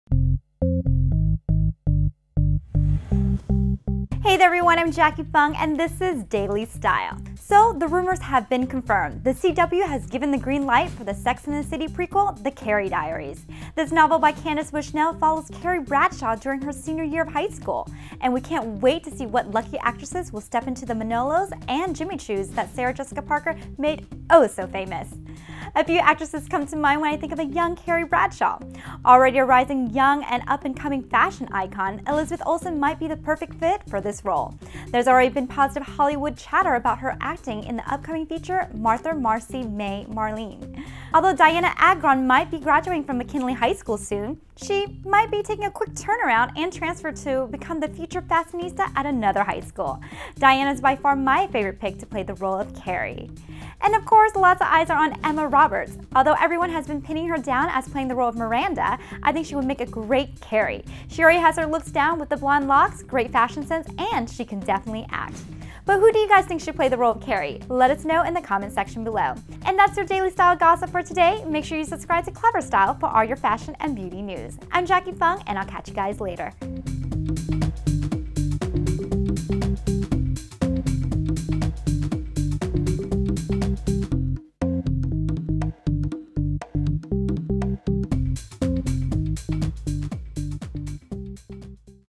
The Carrie Diaries: Dianna Agron Emma Roberts Elizabeth Olsen - the Next Carrie Bradshaw?
The Carrie Diaries: Dianna Agron Emma Roberts Elizabeth Olsen as the Next Carrie Bradshaw?

Hi there everyone! I'm Jacqui Phung and this is Daily Style.

So the rumors have been confirmed. The CW has given the green light for the "Sex and the City" prequel "The Carrie Diaries."

This novel by Candace Bushnell  follows Carrie Bradshaw during her senior year of high school. And we can't wait to see what lucky actresses will step into the Manolos and Jimmy Choos that Sarah Jessica Parker made oh so famous.

A few actresses come to mind when I think of a young Carrie Bradshaw.
Already a rising young, up and coming fashion icon, Elizabeth Olsen might be a perfect fit for the role.  There's already been positive Hollywood chatter about her acting in the upcoming feature "Martha Marcy May Marlene."

Although Dianna Agron might be graduating from McKinley High School soon but she might make a quick turnaround or transfer and become the future fashionista at another school. Dianna is by far my favorite pic to play the role of Carrie.

And of course, lots of eyes are on Emma Roberts. Although everyone has been pinning her down as playing the role of Miranda, I think she would make a great Carrie. She already has her looks down with the blonde locks, great fashion sense and she can definitely act!

But who do you guys think should play the role of Carrie?

And that's you Daily Style gossip for today. I'm Jacqui Phung and I'll see yall later!